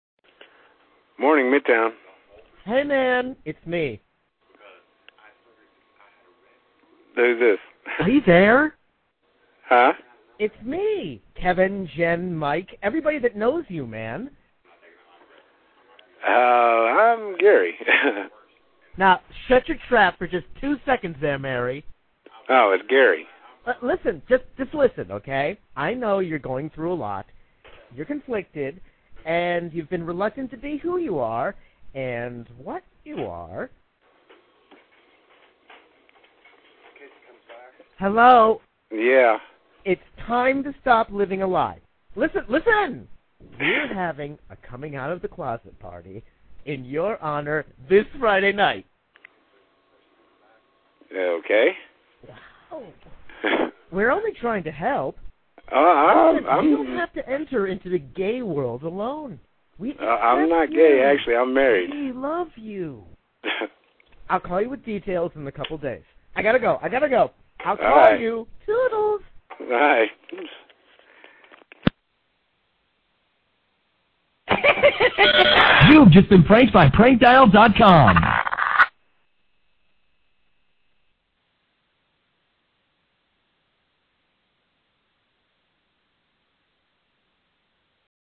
Coming Out Of The closet Prank
Little does Gary Know, That mary is his Secret Name.